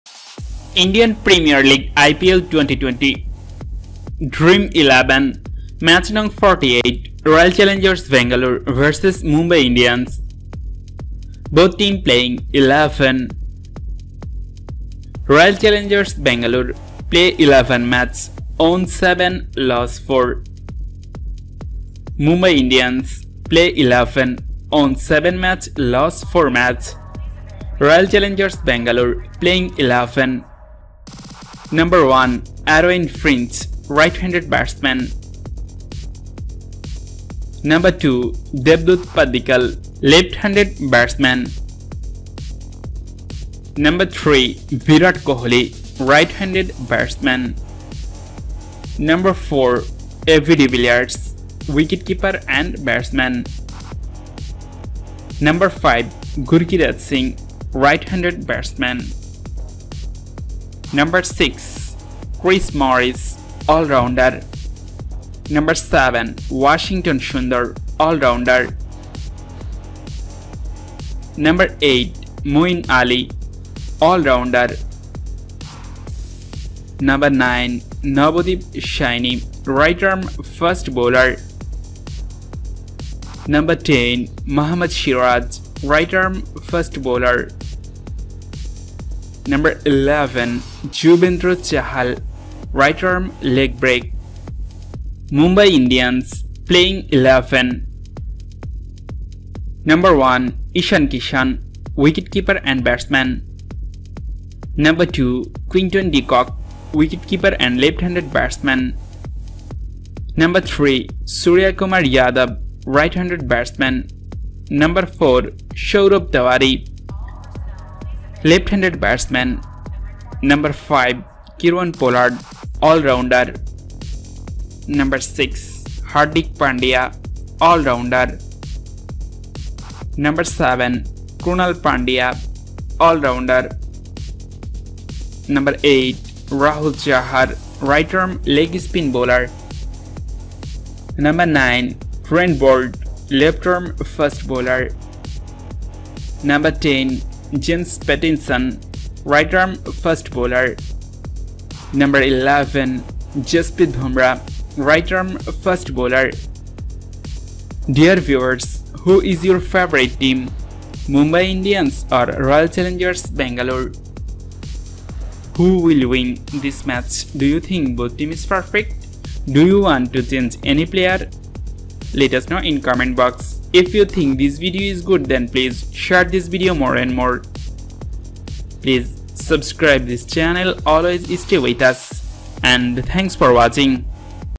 Dream11 Ipl Match no 48 | Bangalore Vs Mumbai Both Teams Playing 11 | Rcb Vs Mi Playing xi
Dream11 Ipl Match no 48 | Bangalore Vs Mumbai Both Teams Playing 11 | Rcb Vs Mi Playing xi#mivsrcb

Indian Premier league Ipl 2020
Dream 11 Ipl Match No: 48,  Rcb vs Mi Both team Playing 11

Rcb Play 11 Match Won 7, Loss 4

Mi Play 11 Won 7, Loss 4

1. Aaron Finch Right-Handed Batsman
2. Devdut padikkal  Leftt-Handed Batsman
3. Virat Kohli Right-Handed Batsman
4. Ab De Villiers wt and Batsman
5. Gurkeerat Singh Right-Handed Batsman
6. Chris Morris All-rounder
7. Washington Sundar All-rounder
8. Moin Ali All-rounder
9. Navdeep Saini Right-arm Fast Bowler
10. Mohammed Siraj Right-arm Fast Bowler
11. Yuzvendra Chahal Right-arm leg break

1.Ishan Kishan Wicket-Keeper and Batsman
2. Quinton De kock Wicket-Keeper and left handed Batsman
3. Suryakumar Yadav Right Handed Batsman
4. Saurabh Tiwary Left Handed Batsman
5. Kieron Pollard All-rounder
6. Hardik pandya All-Rounder
7. Krunal Pandya All-rounder
8. Rahul Chahar Right-arm Leg Spin Bowler
9. Trent Boult Left-arm Fast Bowler
10. James Pattinson Right-arm Fast Bowler
11. Jasprith Bumrah Bowler From India